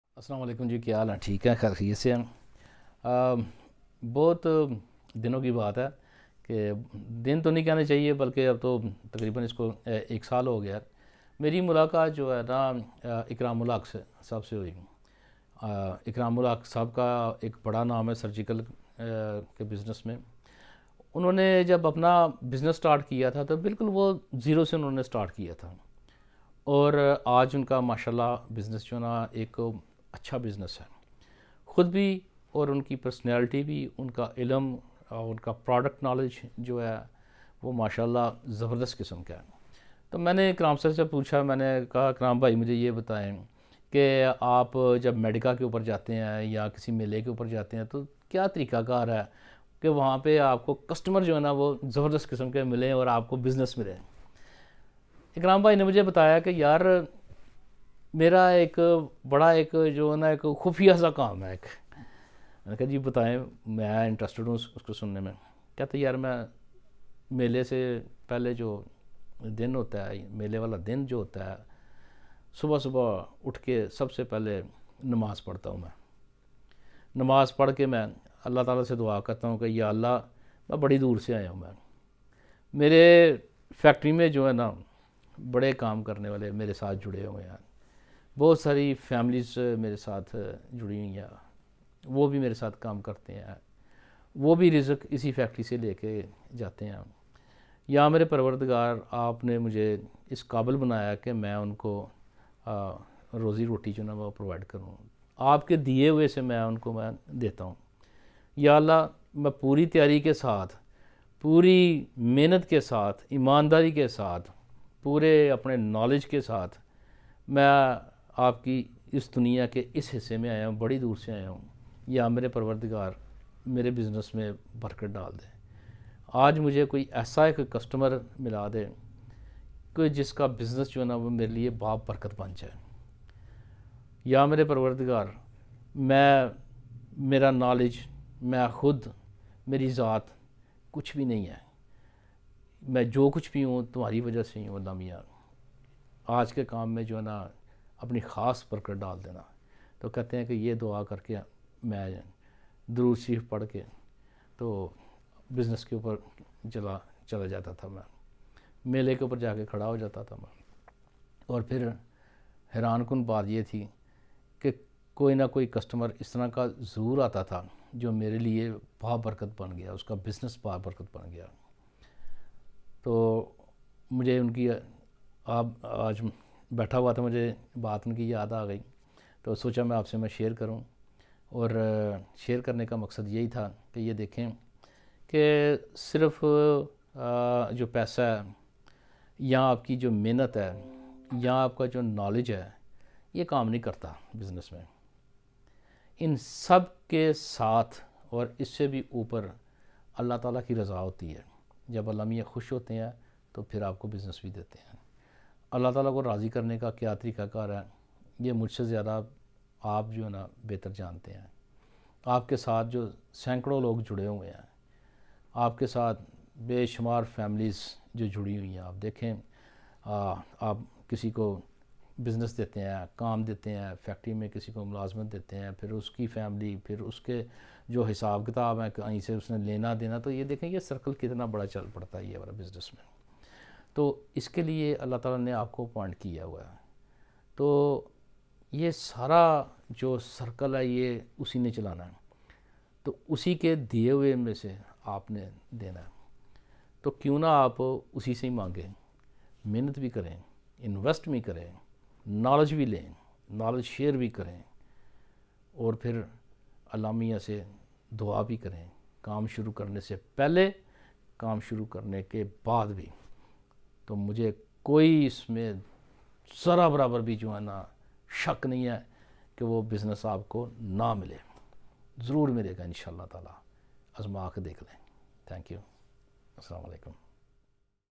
How to Start your business day. Ikram-Ul-Haq. Former Senior Vice Presidents Sialkot Chamber (SCCI)
Mr. Ikram-Ul-Haq
M/s:IAA Surgical Instruments Co
Toorabad Daska Road, Sialkot.